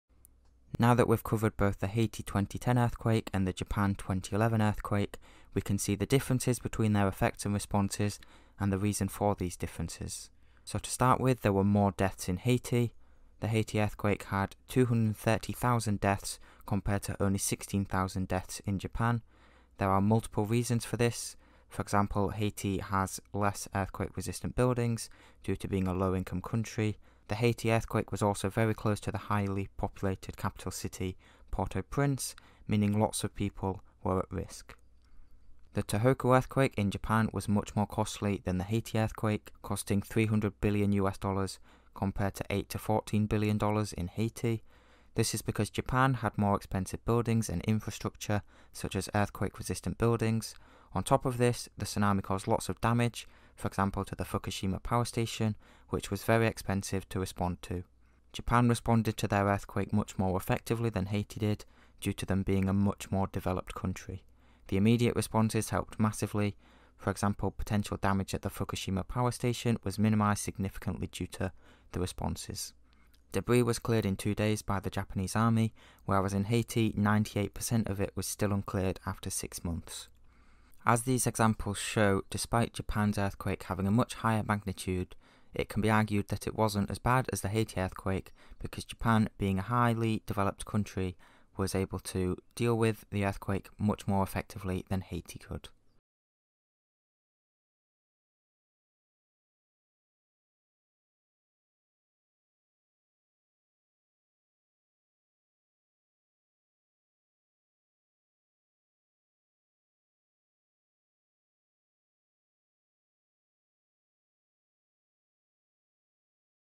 Comparing Earthquakes (Haiti 2010 + Japan 2011) | AQA GCSE 9-1 Geography
Comparing the Haiti 2010 and Japan 2011 earthquakes.

This is the seventh video for the AQA GCSE 9-1 Geography course, and the seventh video of the Challenge of Natural Hazards topic.

Do you have a video suggestion?

Video script:
Now that we have covered both the Haiti 2010 earthquake and Japan 2011 earthquake, we can see the differences between their effects and responses, and the reasons for those differences.

More deaths in Haiti:
The Haiti earthquake had 230,000 deaths compared to only 16,000 in Japan.
There are multiple reasons for this, including Haiti having less earthquake resistant buildings due to being a low-income country. The Haiti earthquake was also very close to the highly populated capital city, meaning lots of people were at risk.

A higher cost in Japan:
The Tohoku earthquake in Japan was much more costly than the Haiti earthquake, costing $300bn compared to $8-14bn in Haiti. This is because Japan had more expensive buildings and infrastructure, such as earthquake resistant buildings. On top of this, the tsunami caused lots of damage, for example to the Fukushima power station, which was very expensive to respond to.

Faster and more effective responses in Japan
Japan responded to their earthquake much more effectively than Haiti did, due to them being a much more developed country. The immediate responses helped massively, for example potential damage at the Fukushima power station was minimised significantly due to them. Debris was cleared in 2 days by the Japanese Army, whereas in Haiti 98% was still uncleared after 6 months.

GCSE Geography
30/04/2020
Keducate